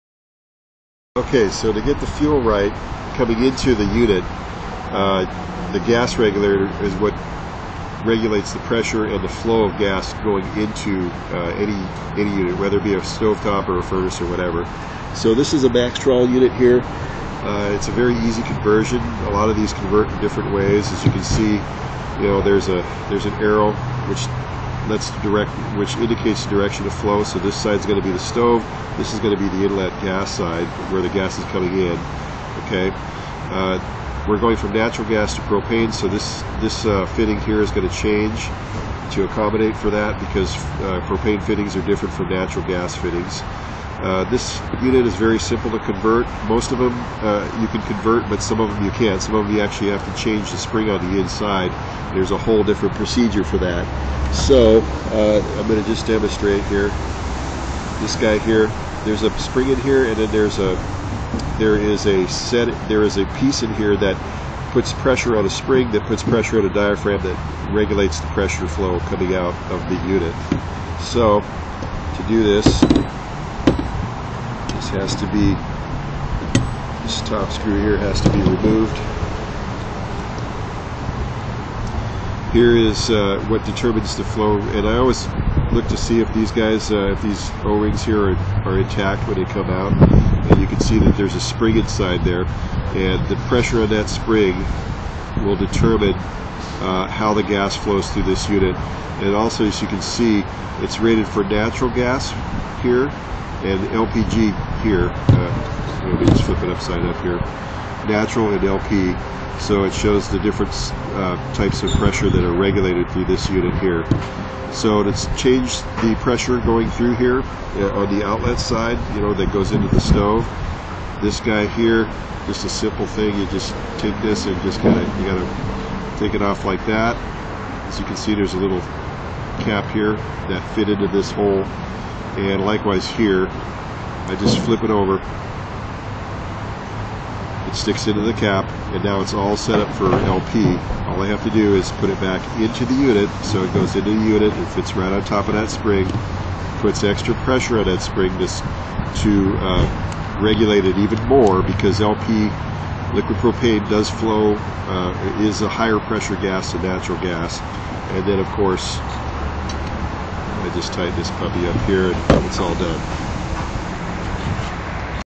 Converting A Maxtrol Gas Regulator for Natural Gas (NG) or Propane (LP)
This video shows how a conversion is done on a Maxtrol Regulator when converting between natural gas (NG) and propane (LP).